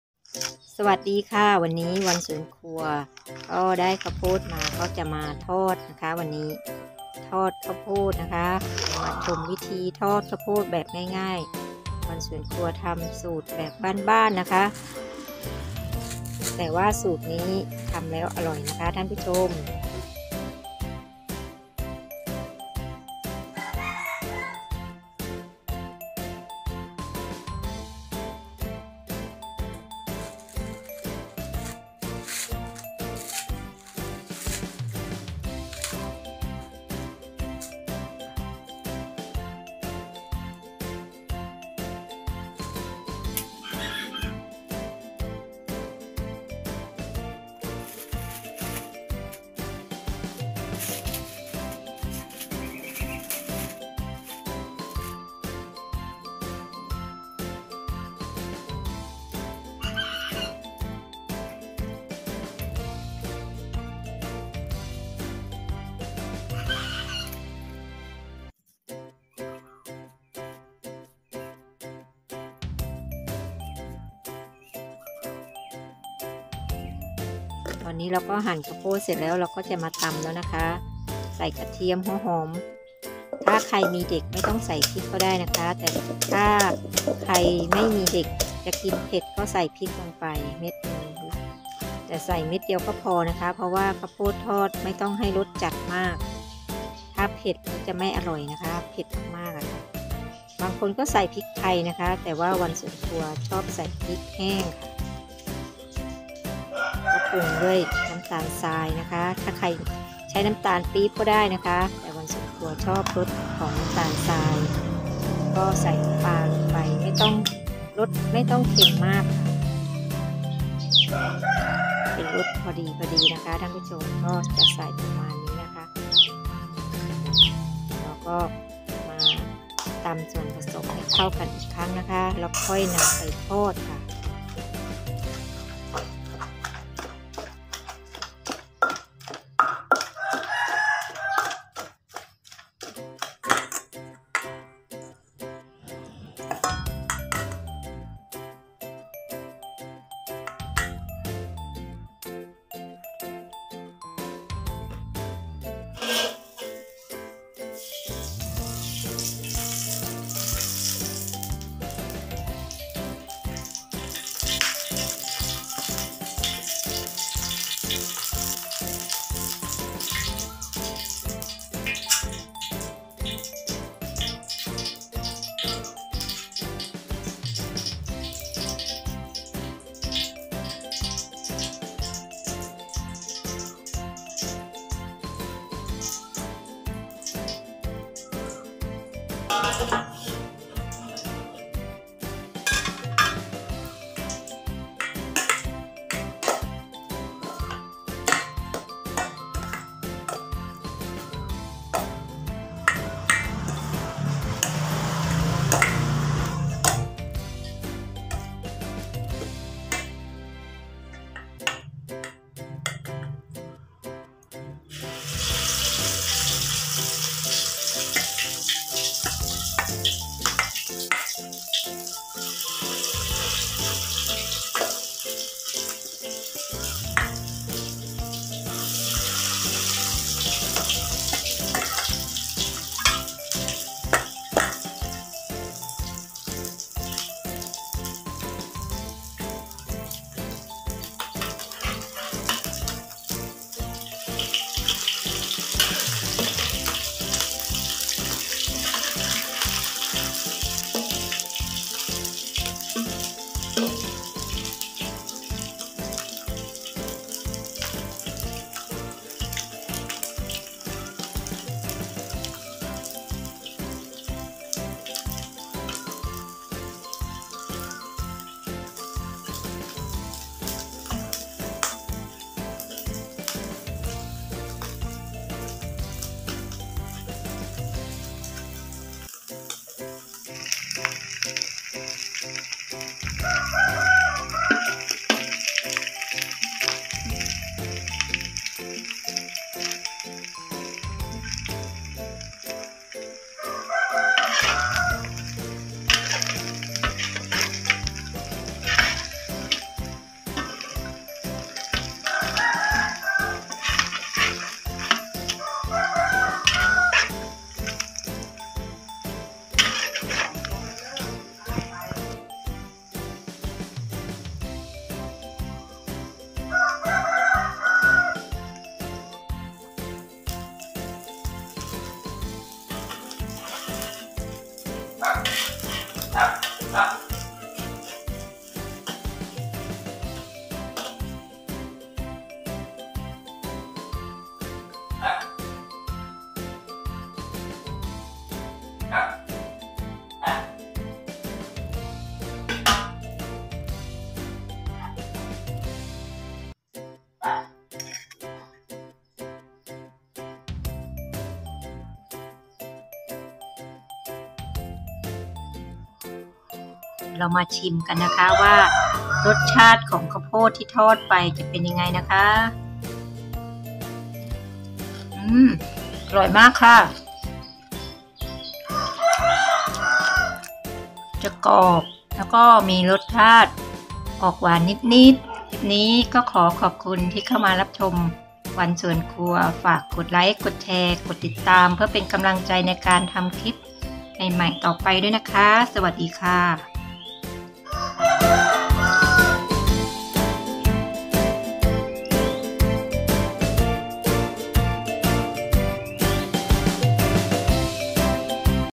ทอดข้าวโพดข้าวเหนียวทำแบบนี้อร่อยมาก
วัฒถุดิบ
ข้าวโพด กระเทียม หัวหอม น้ำปลา พริกน้ำตาล
ตำกระเทียม หัวหอม พริก ข้าวโพดแล้วปรุงรสด้วยน้ำตาลน้ำปลา